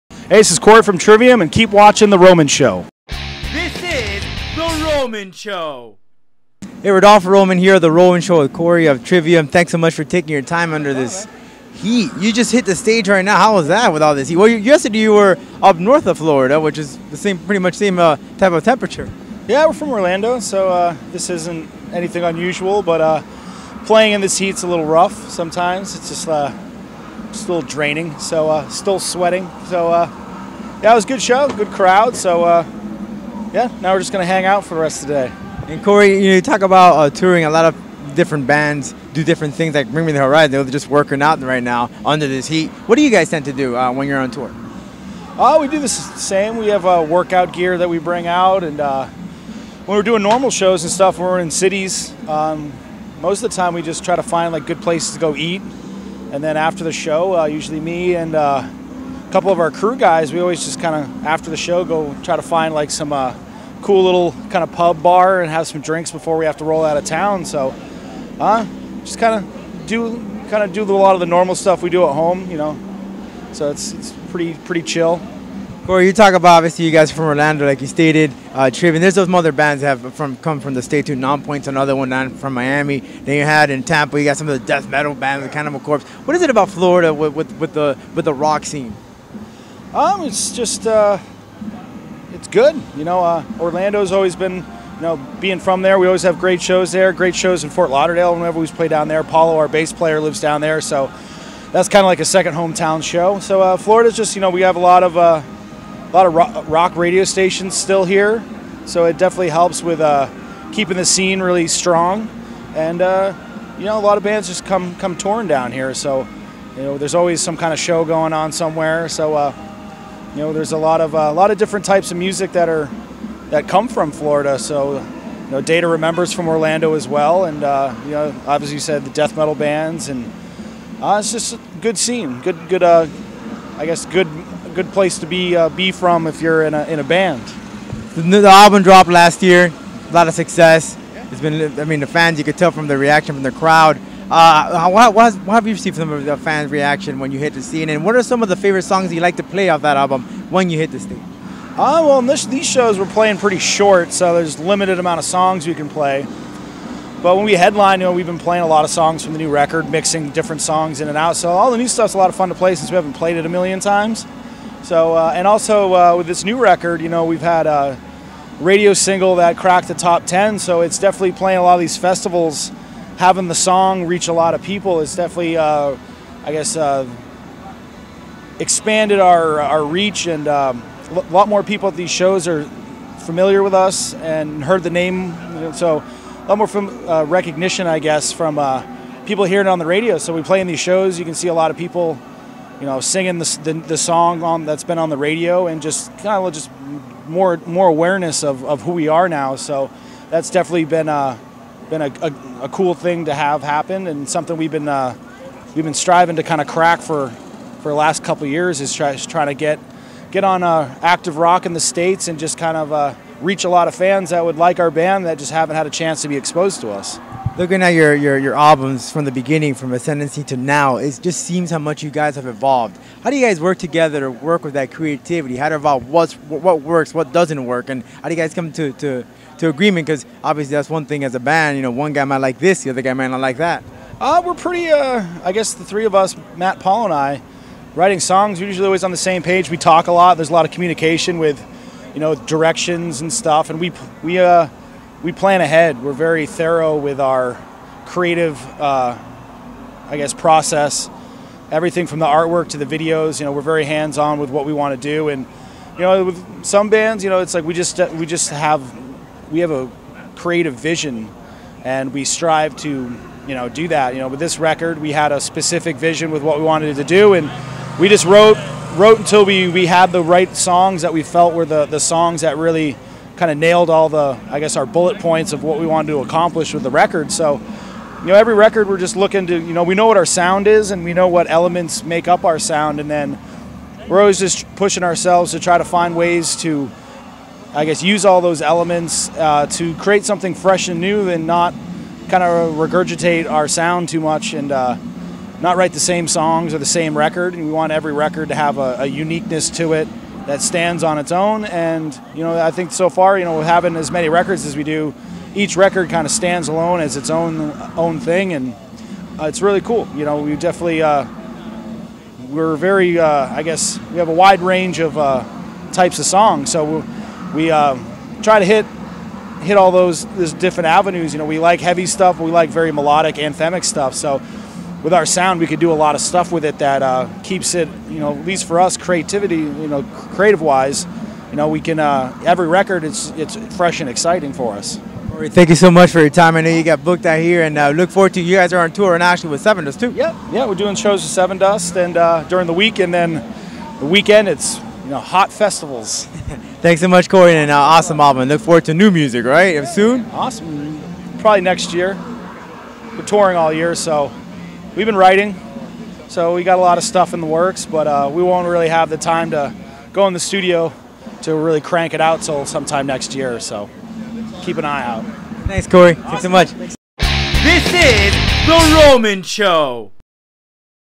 Corey King Beaulieu of Trivium Talks Creative Process, Touring New Music
Lead guitarist Corey King Beaulieu of Trivium, based out of Orlando, talked the Florida metal scene, new music, what songs the band plays on tour from the new album and the creative process.